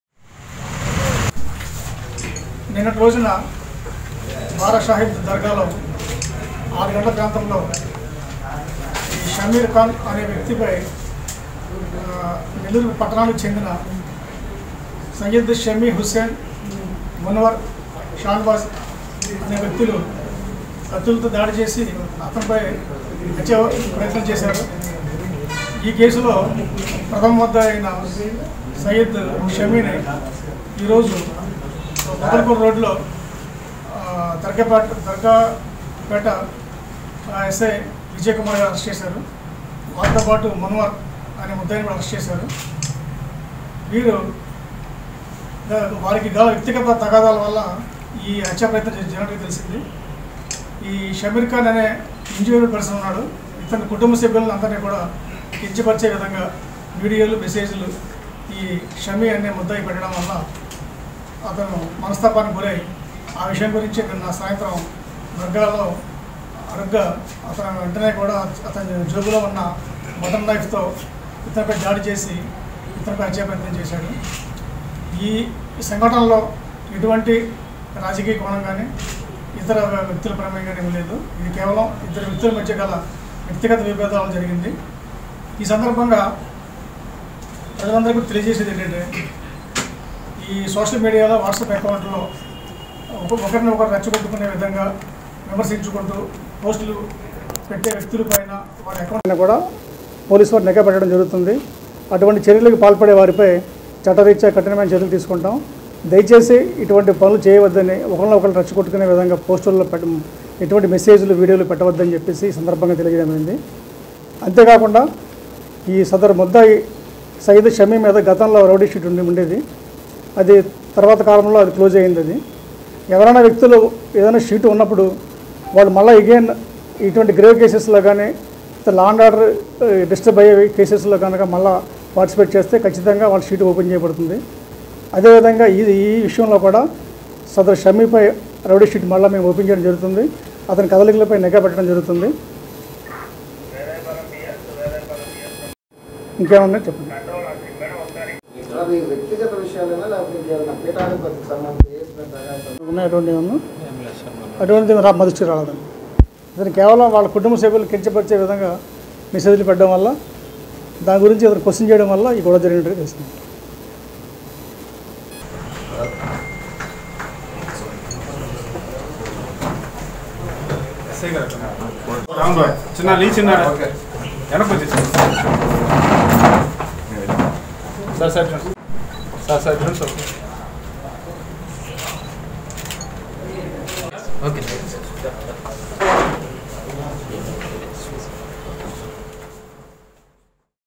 నెల్లూరులో సమీర్ పై హత్యాయత్నం కేసులో సమీ అరెస్ట్ ,
నెల్లూరు బారాషాహిద్ దర్గాలో మంగళవారం సమీర్ పై జరిగిన హత్యాయత్నం కేసులో , నిందితుడు సమీ , మున్వర్లను పోలీసులు అరెస్ట్ చేశారు. వ్యక్తిగత తగాదాలే ఈ ఘటనకు కారణమని డిఎస్పీ శ్రీనివాసరెడ్డి చెప్పారు. సమిపై రౌడీషీట్ కూడా ఓపెన్ చేస్తామన్నారు.. ► Download NDN Android App